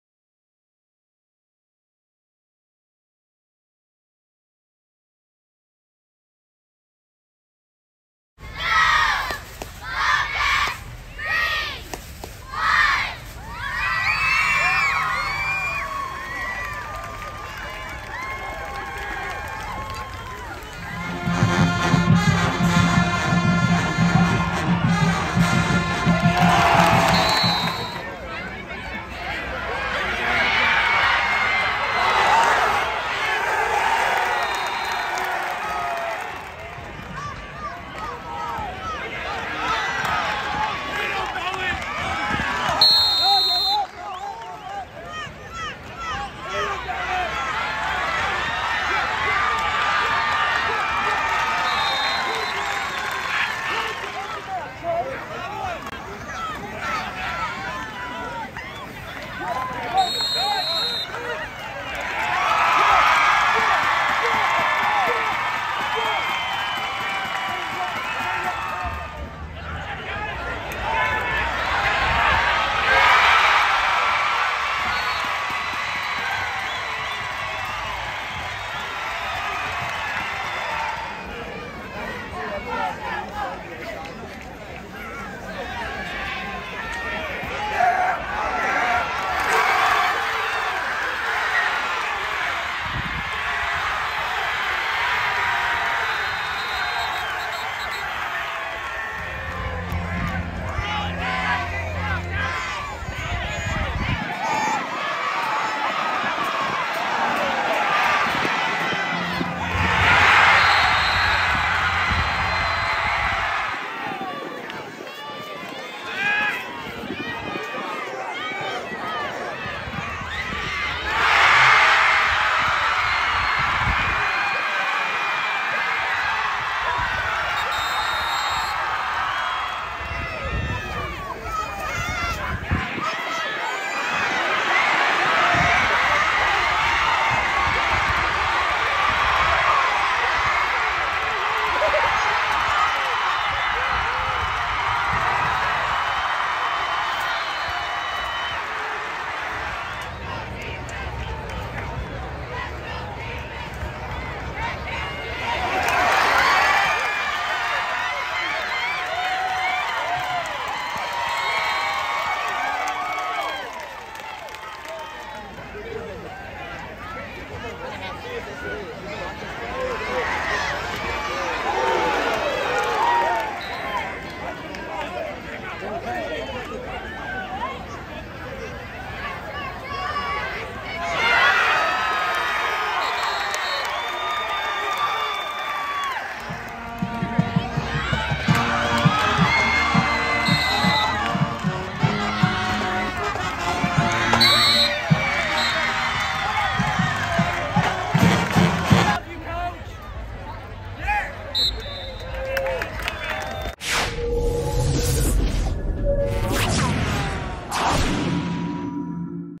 Week 1 Highlights: Springfield at Greenbrier
Highlights from Springfield's 35-0 victory at Greenbrier on Friday, Aug. 19, 2022.

Video by Annette Burton.